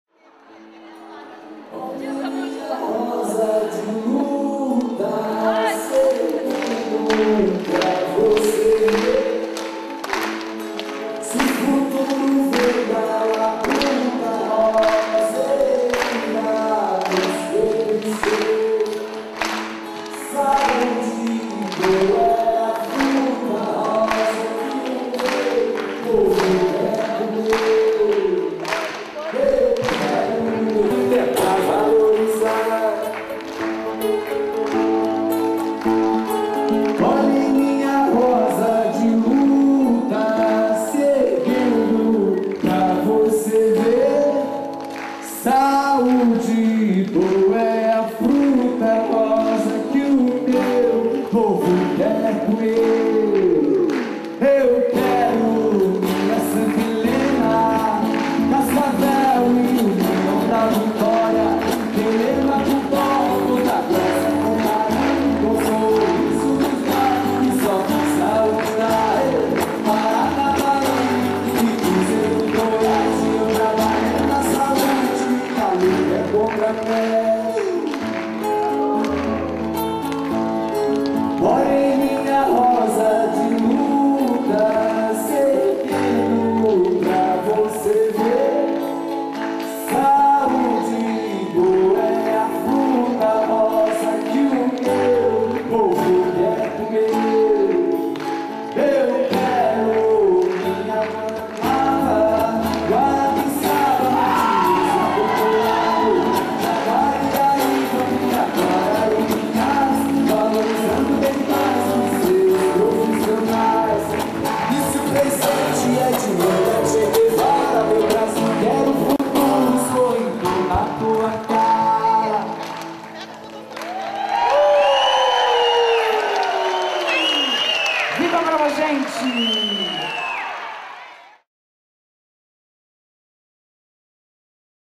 Mobilizações da brava gente em 2012
Imgens das mobilizações da brava gente da saúde em 2012. SindSaúde-PR